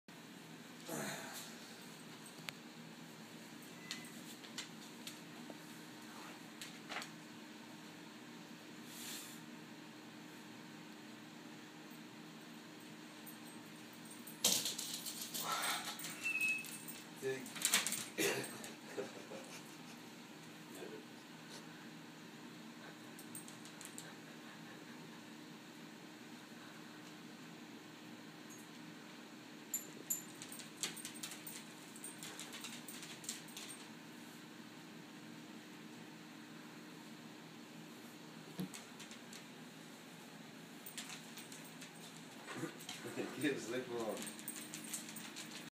Mom's Cat Catching a Moth
ahah Mouse catching a moth while mom's fiance helps him out